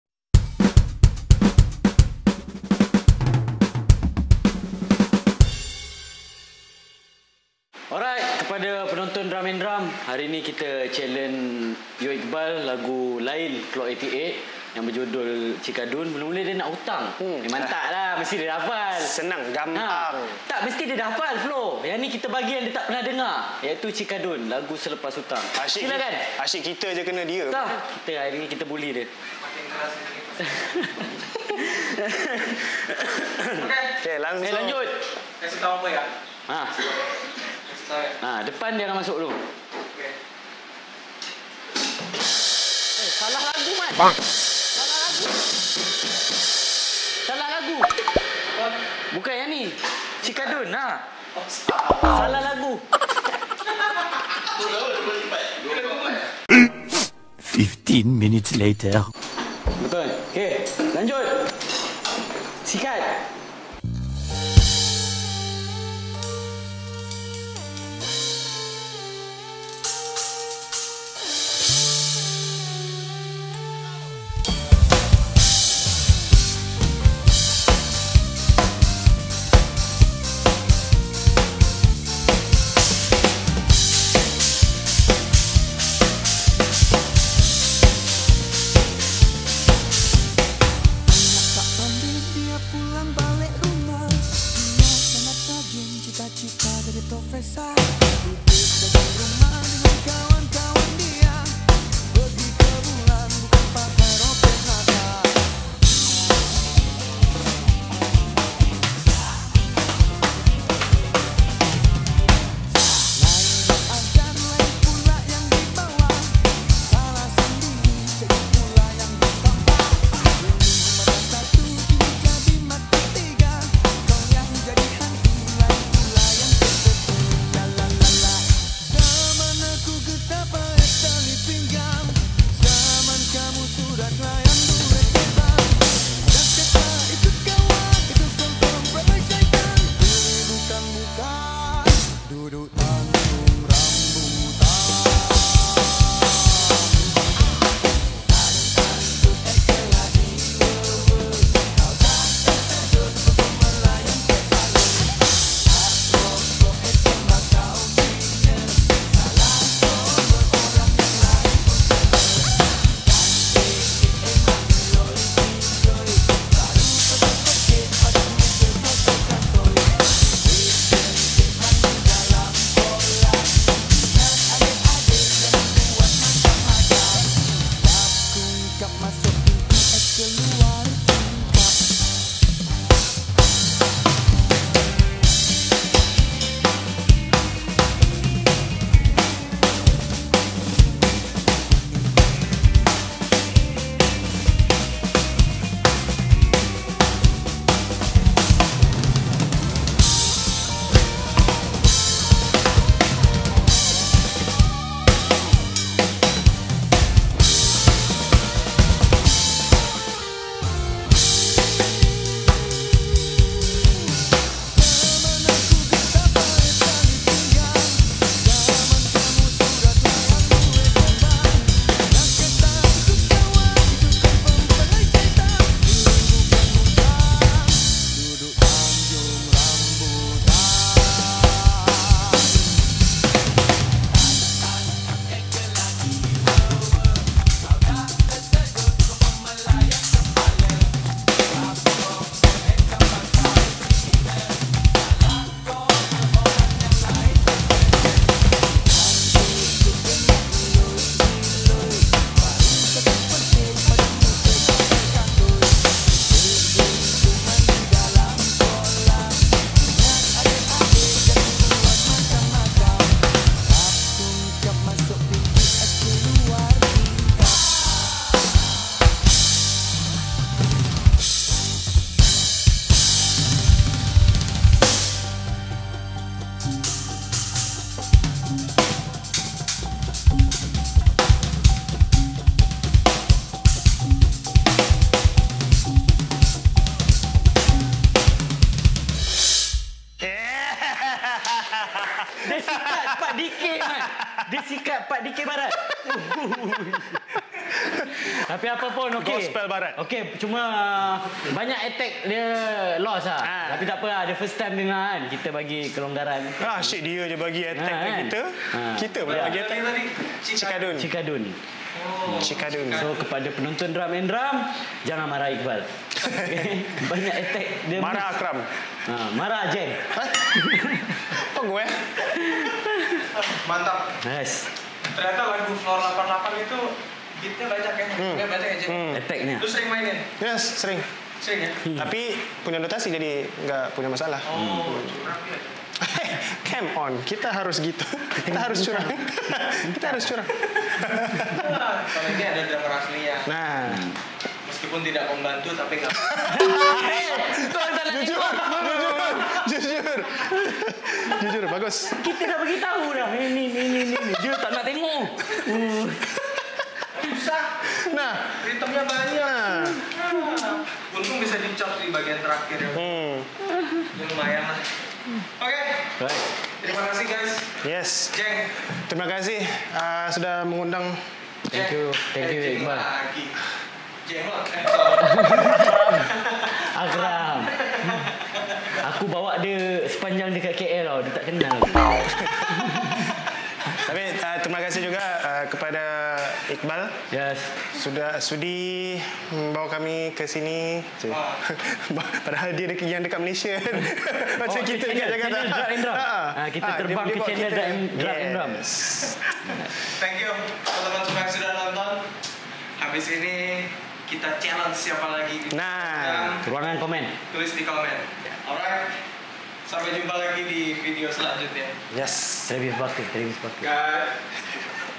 EP #4 - Giliran YoIqbal drumndrum pula dibuli oleh Akram & Jenk!!!
Video/Audio Editing : MYdrum Team(zaidie_roland & akramf88)
Location : Odotz Music House, Klang